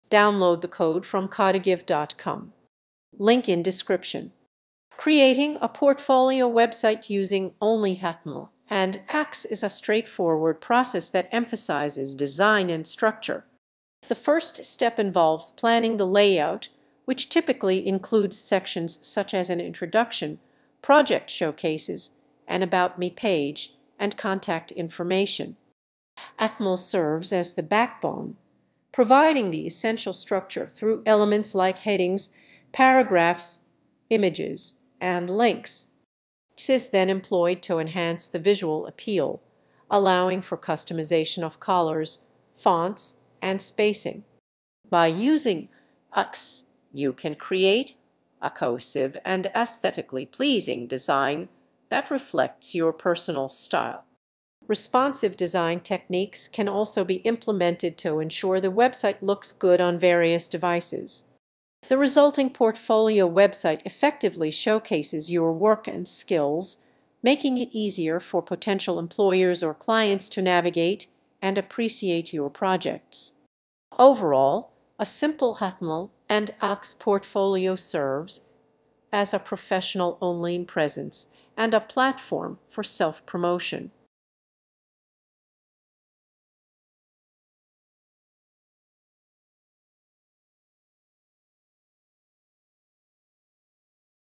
Create a portfolio website only using html and css
creating a portfolio website using only html and css is a straightforward process that emphasizes design and structure. the first step involves planning the layout, which typically includes sections such as an introduction, project showcases, an about me page, and contact information. html serves as the backbone, providing the essential structure through elements like headings, paragraphs, images, and links.

css is then employed to enhance the visual appeal, allowing for customization of colors, fonts, and spacing. by using css, you can create a cohesive and aesthetically pleasing design that reflects your personal style. responsive design techniques can also be implemented to ensure the website looks good on various devices.

the resulting portfolio website effectively showcases your work and skills, making it easier for potential employers or clients to navigate and appreciate your projects. overall, a simple html and css portfolio serves as a professional online presence and a platform for self-promotion.

python create class
python create requirements.txt
python create folder
python cssselect